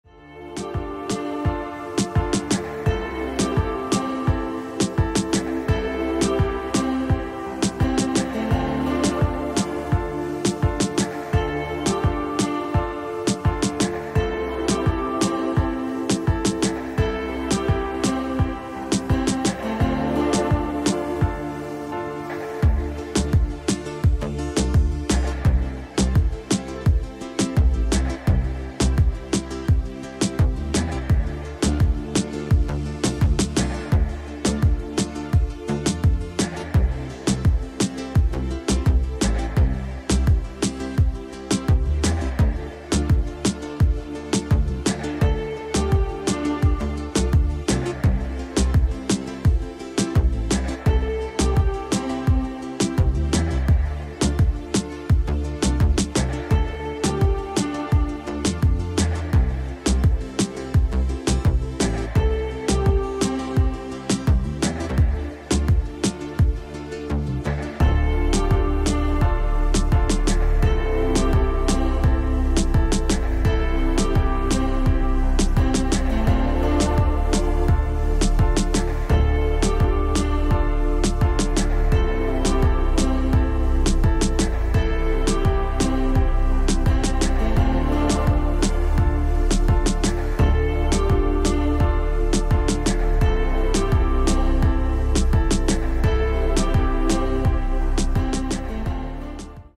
SCANIA R450 - Cartwright Bros Haulage Ltd - A1(M) motorway trucks spotting
Mercedes-Benz
VOLVO FH - British Gypsum
 SCANIA R450 - Cartwright Bros Haulage Ltd